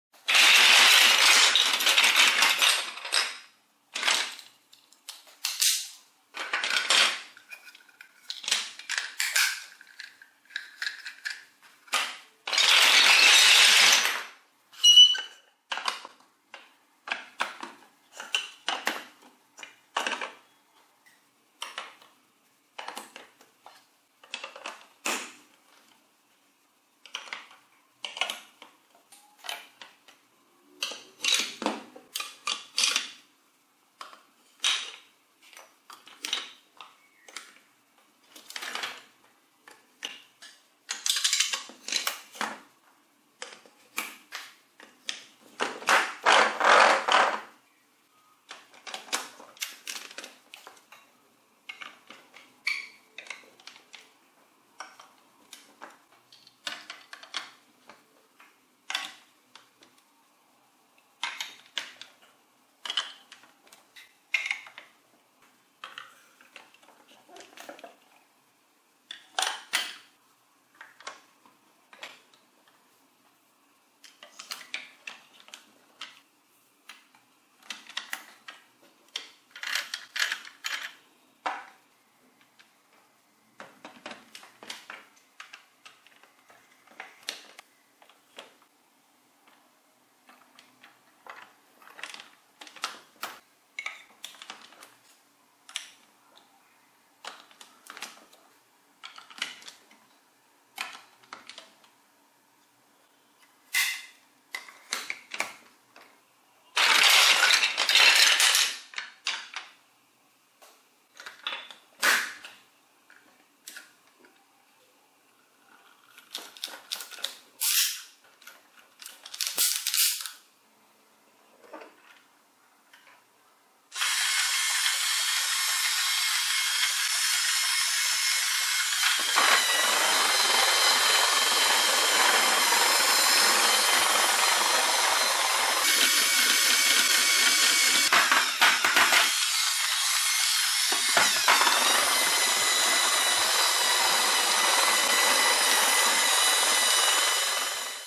JCB Excavator Satisfying DIY vehicle building with blocks ビルディングブロック
I am making diy vehicle video using blocks
レゴ デュプロと互換性のあるブロックを使用して DIY の乗り物を組み立てます